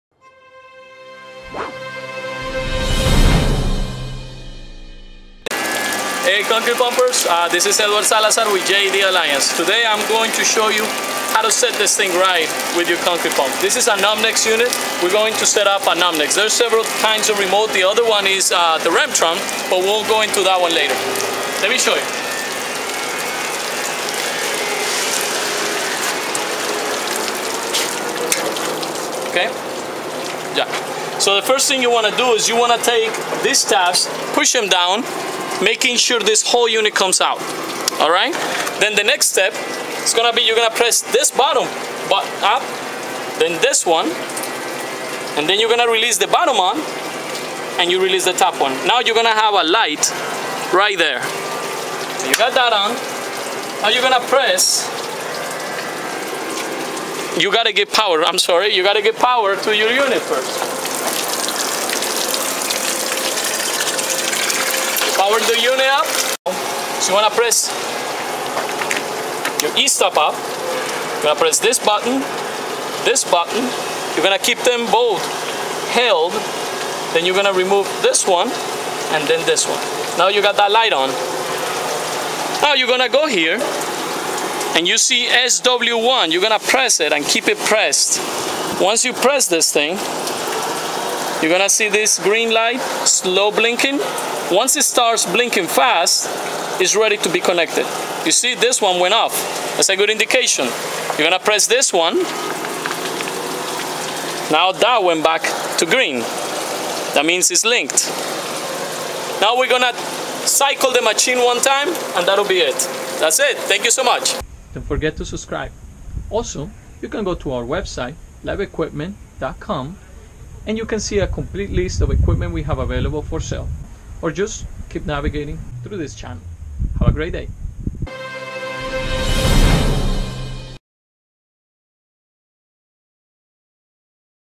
Testing an OMNEX Concrete Pump Remote | Tutorial
you will learn how to properly set up your wireless omnex remote control, these can be used in a wide variety of concrete pumps

JED Alliance Group has a large inventory of used concrete mixer trucks and concrete pumps for sale.